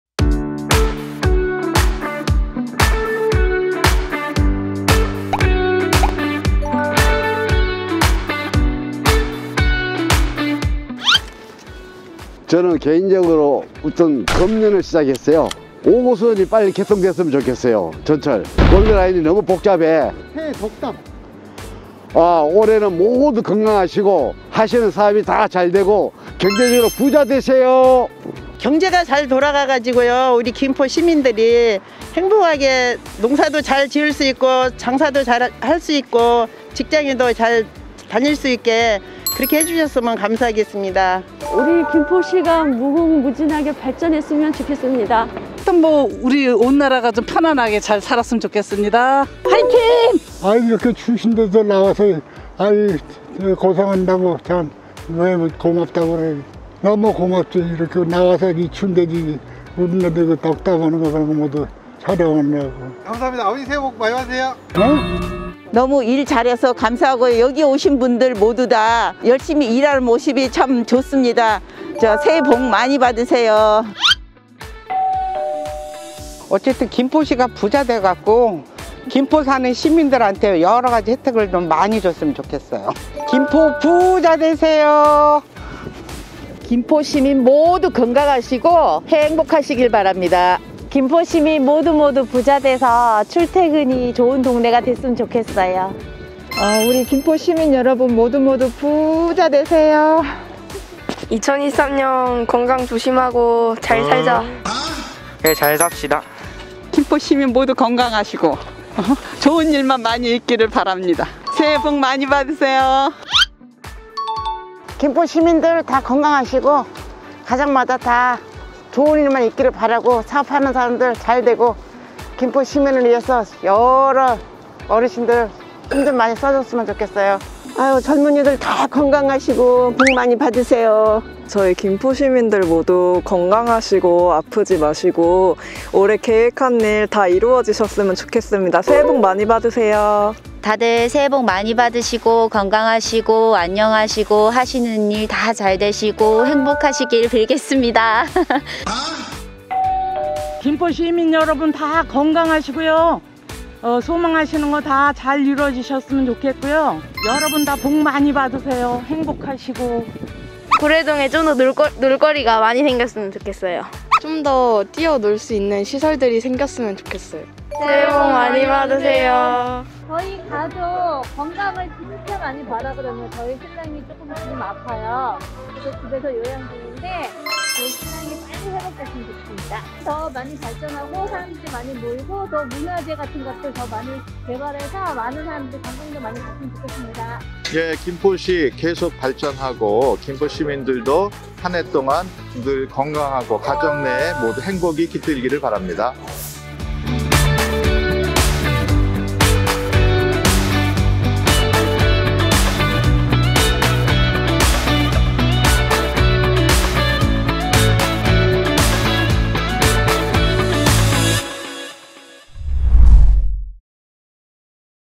💖통하는 김포! 2023년 새해맞이 시민 덕담 릴레이🎤💖
2023년 계묘년, 본격 새해맞이 시민 인터뷰!
2023년 새해, 김포시에 대한 시민분들의 생각은 어떠할지
시민 인터뷰를 진행하였습니다.

김포시에 대한 시민분들의 많은 기대와 응원을 만나볼 수 있었는데요~
영상에서 시민분들의 덕담을 함께 들어보시죠!